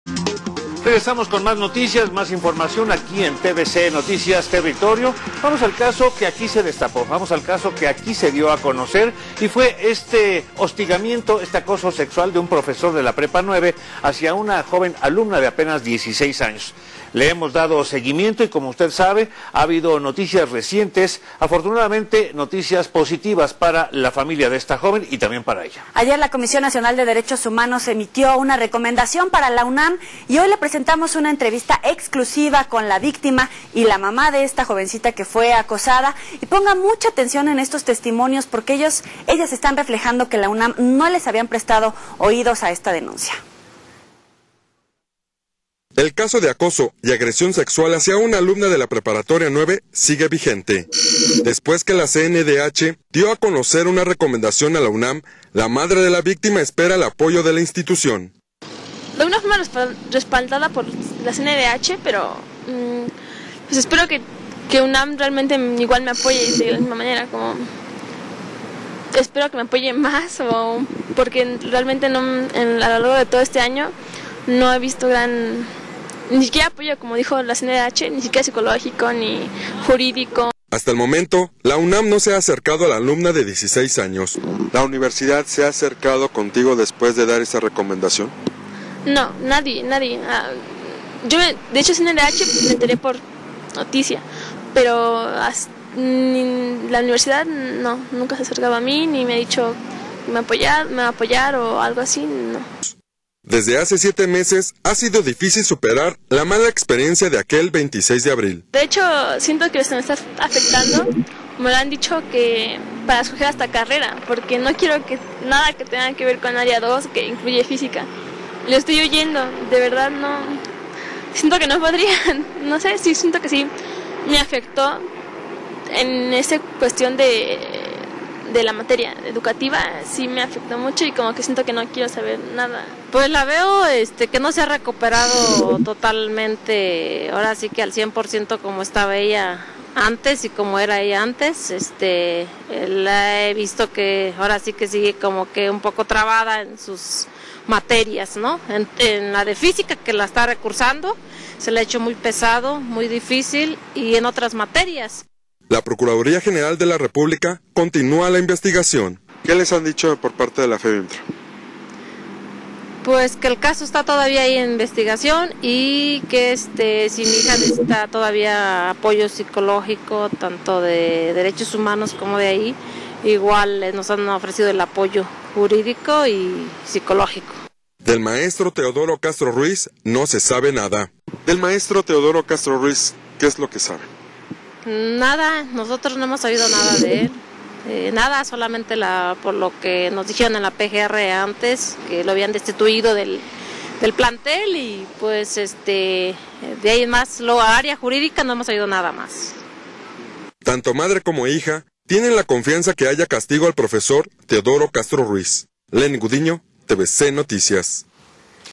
Alumna acosada por profesor de la Prepa 9 habla en exclusiva por resolución de la CNDH
El caso de acoso y agresión sexual hacia una alumna de la preparatoria nueve sigue vigente... Luego que la CNDH dio  a conocer una recomendación a UNAM, la madre de la victima espera el apoyo de la institución...